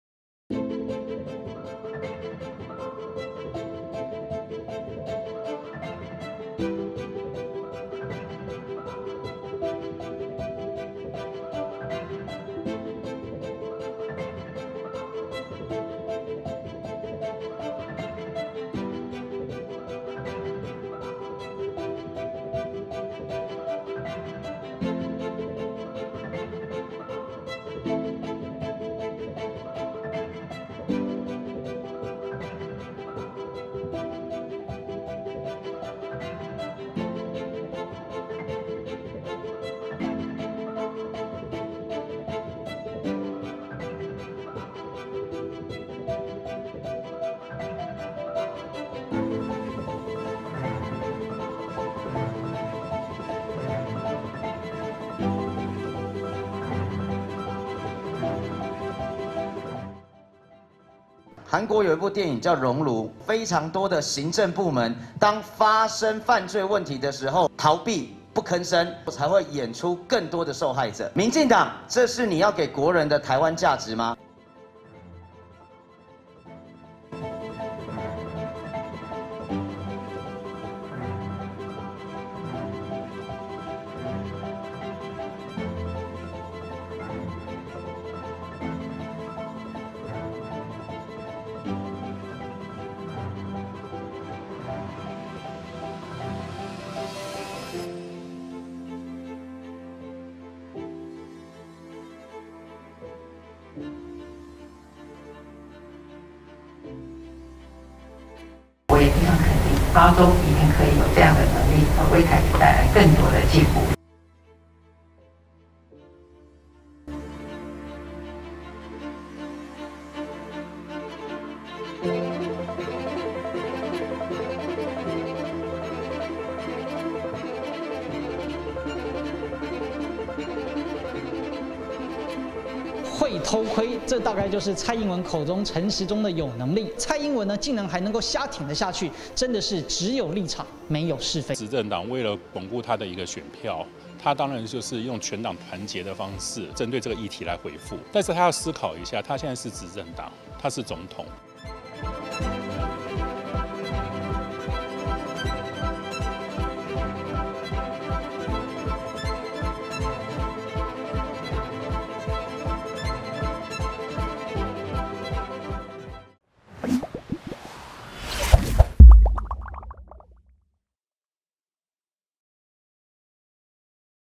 總統在瞎挺？！陳時中「廁所偷窺片」惹議 侯漢廷：只有立場沒有是非 @ChinaTimes​
民進黨台北市長參選人陳時中宣傳公廁裝免治馬桶，影片中竟出現他在廁所偷窺的畫面，外界砲轟類似韓國校園霸凌電影"熔爐"的橋段，根本把性騷當有趣。但蔡總統出席活動時，還大讚力挺陳時中有能力，在野黨砲轟從林智堅抄襲，到陳時中的偷窺片惹議，蔡英文根本是瞎挺，只有立場不管是非。